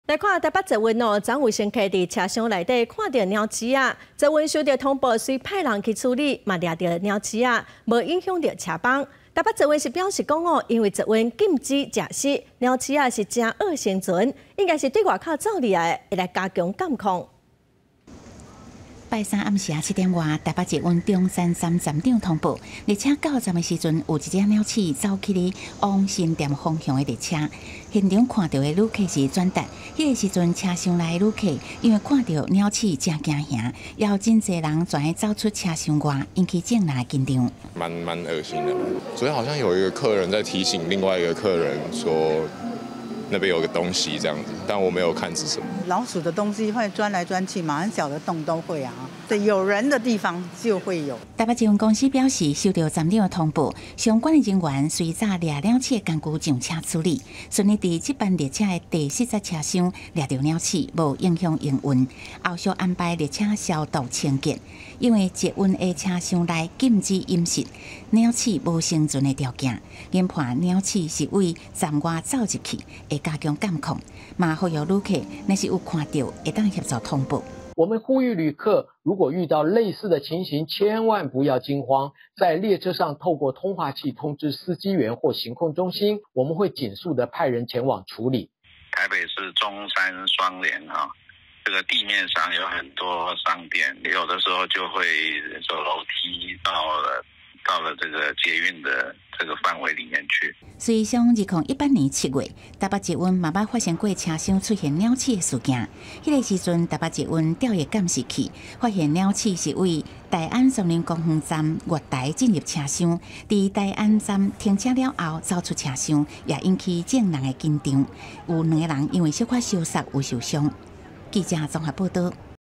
中山站老鼠上車引恐慌 北捷獲報捕獲並安排清消｜20240822 公視中晝新聞
台北捷運車廂昨（21）日有乘客驚見老鼠，引起一陣騷動，北捷收到通報後立刻派員上車處理，順利捕獲老鼠，並未影響營運。北捷表示，捷運系統站內禁止飲食，老鼠並沒有生存條件，研判從站外進入，將加強監控。